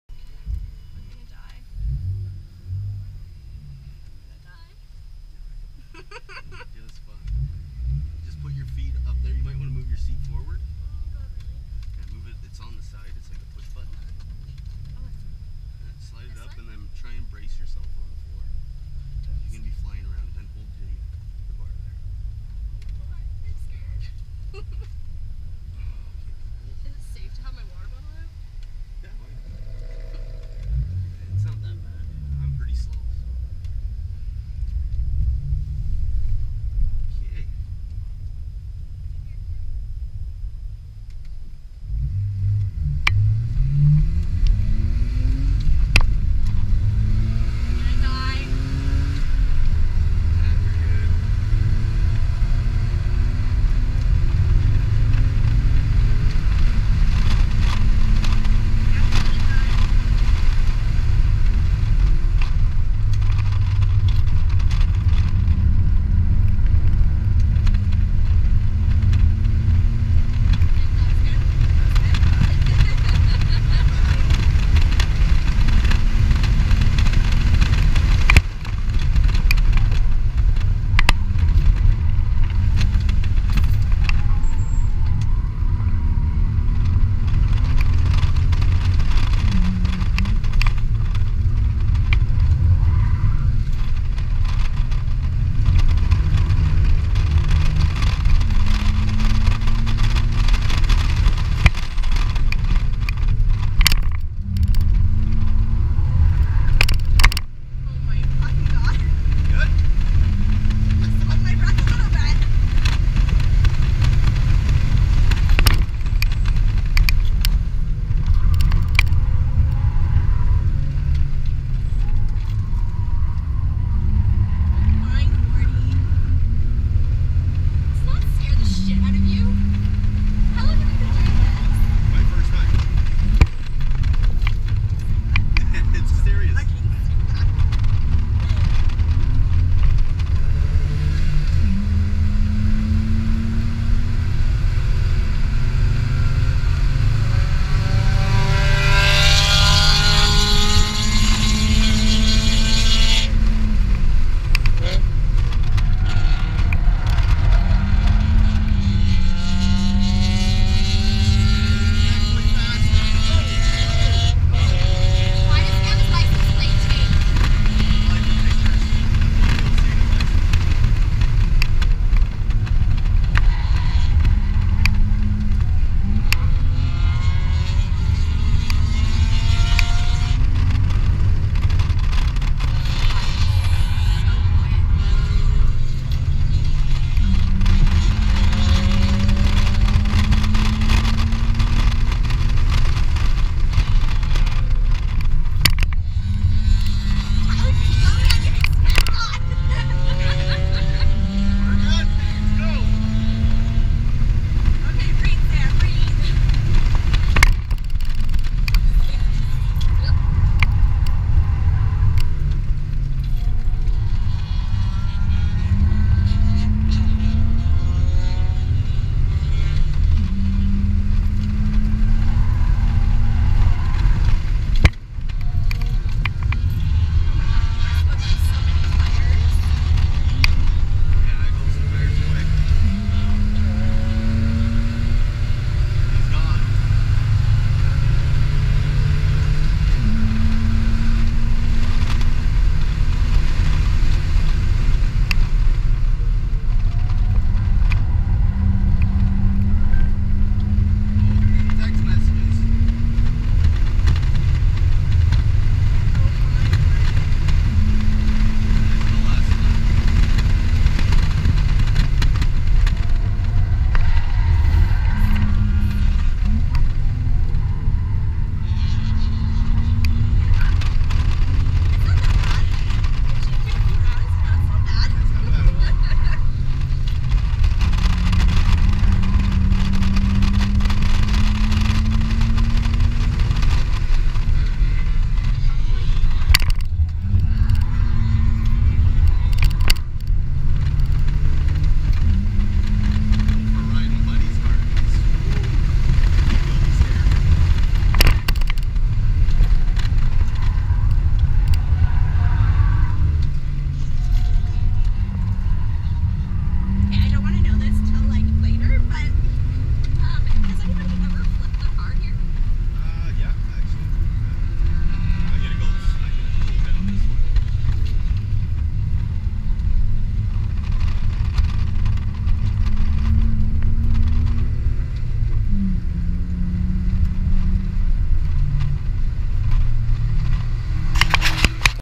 E46 TRACK DAY. Toronto Motorsport Park Cayuga
First ever track day at TMP Cayuga.
2003 BMW 325i
Tein s tech lowering springs
Hawk hp plus pads
Hankook Rs4 tires